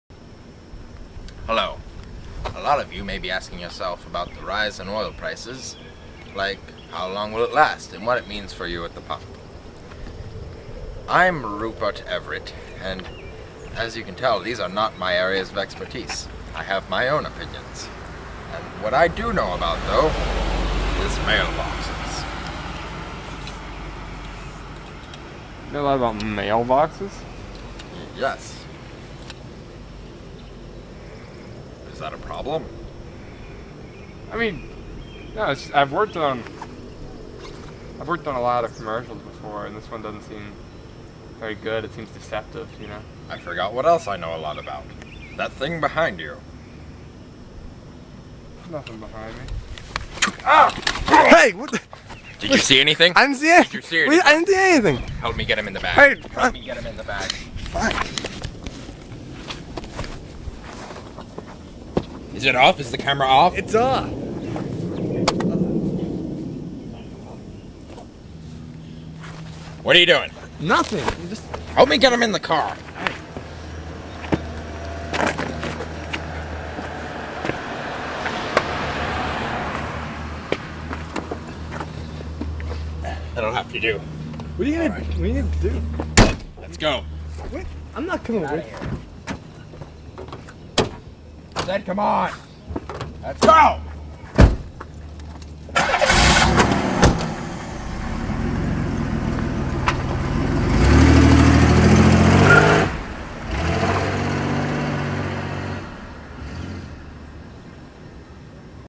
The Mailbox Factory
You know how that Original Matress Factory guy always wants to talk about weird stuff and then is like "oh yeah by the way buy a matress."? It's about that. parody